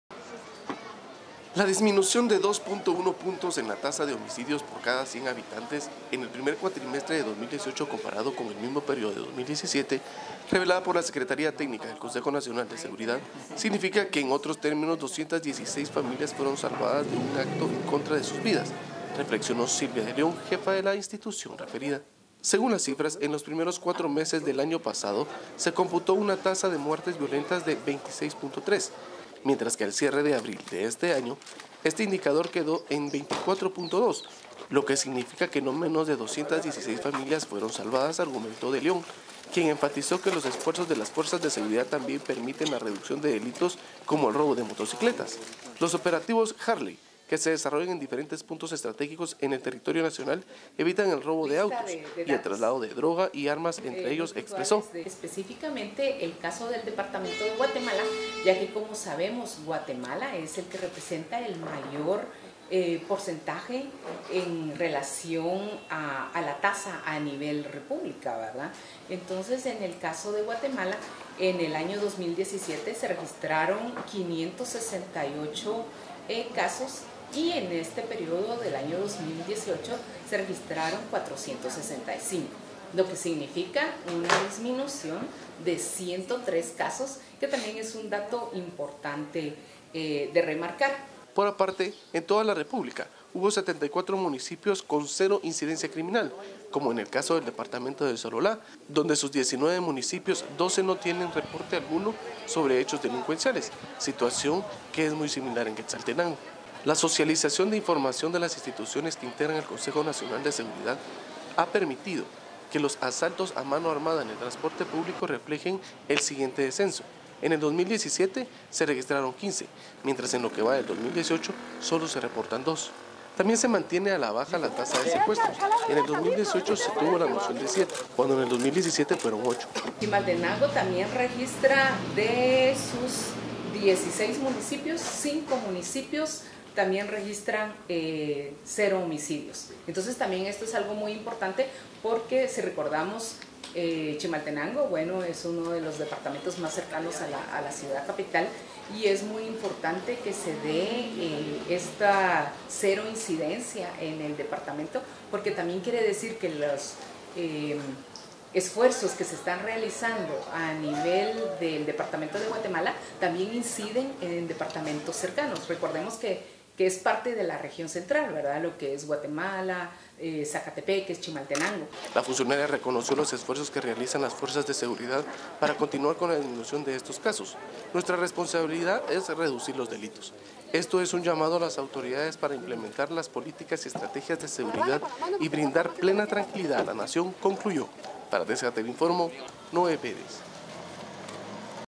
Disminución de homicidios en primer cuatrimestre de 2018 significa no menos de 216 familias salvadas
La disminución de 2.1 puntos en la tasa de homicidios por cada 100 mil habitantes en el primer cuatrimestre de 2018 comparado con el mismo periodo de 2017, revelada por la Secretaría Técnica del Consejo Nacional de Seguridad, significa en otros términos que 216 familias fueron salvadas de un acto en contra de sus vidas, reflexionó Silvia de León, jefa de la instancia referida.
Según las cifras, el los primeros cuatro meses del año pasado se computó una tasa de muertes violentas de 26.3, mientras que al cierre de abril de este año este indicador quedó en 24.2, lo que “significa que no menos de 216 familias fueron salvadas”, argumentó De León, quien enfatizó que los esfuerzos de las fuerzas de seguridad también permiten la reducción de delitos como el robo de motocicletas.
Los operativos Harley que se desarrollan en diferentes puntos estratégicos en el territorio nacional evitan el robo de estos autos, y el traslado de droga y armas en ellos”, expresó.
74 municipios sin crimen
Por aparte, en toda la República hubo 74 municipios con cero incidencia criminal, como en el caso del departamento de Sololá, donde de sus 19 municipios 12 no tienen reporte alguno sobre hechos delincuenciales, situación que es muy similar en Quetzaltenango.
La socialización de información de las instituciones que integran el Consejo Nacional de Seguridad ha permitido que los asaltos a mano armada en el transporte público reflejen el siguiente descenso: en el 2017 se registraron 15, mientras en lo que va del 2018 solo hay reporte de 2.
También se mantiene a la baja la taza de secuestros. En 2018 se tuvo noción de 7, cuando en 2017 fueron 8.
La funcionaría reconoció los esfuerzos que realizan las fuerzas de seguridad para continuar con la disminución de estos casos. “Nuestra responsabilidad es reducir los delitos. Este es un llamado a las autoridades para implementar las políticas y estrategias de seguridad y brindarle plena tranquilidad a la nación”, concluyó.